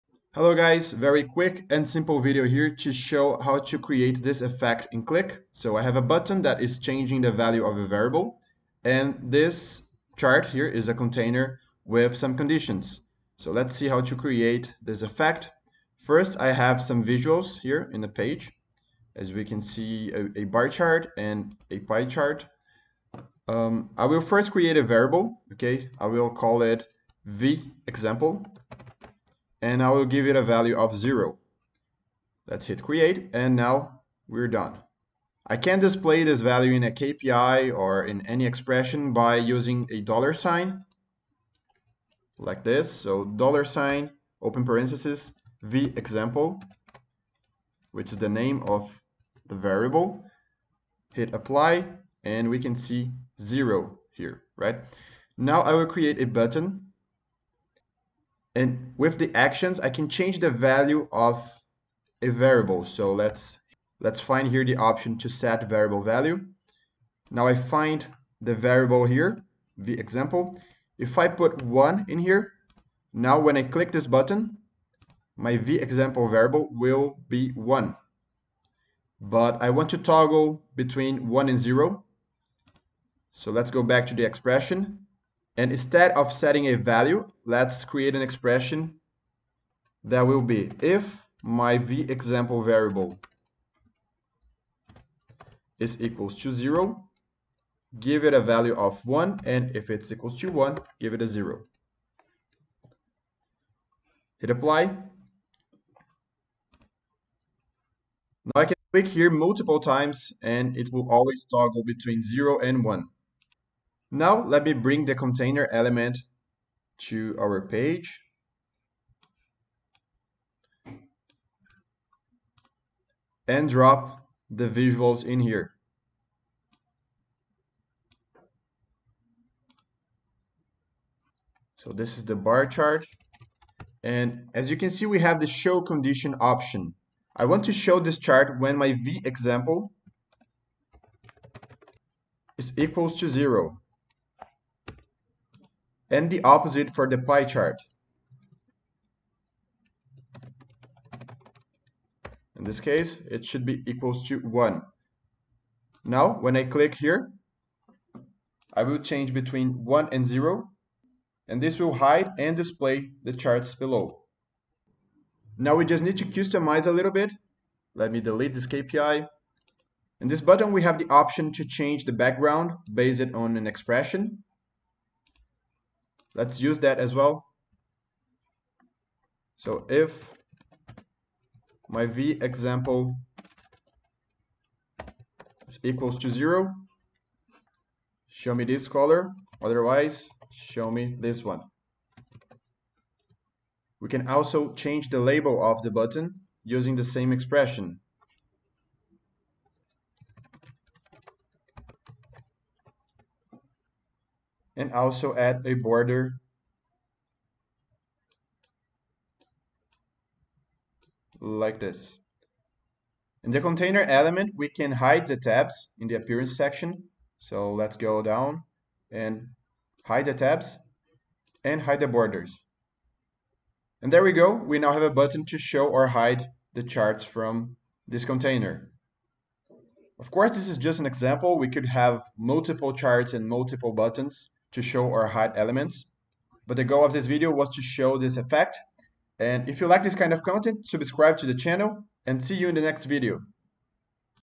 Qlik Sense - Must Know Tips #1 | Using variables to display or hide elements
Ready to learn more about qlik?
and check out the other videos here in the channel as well!

How to use the container element with variables in Qlik and save space in your page.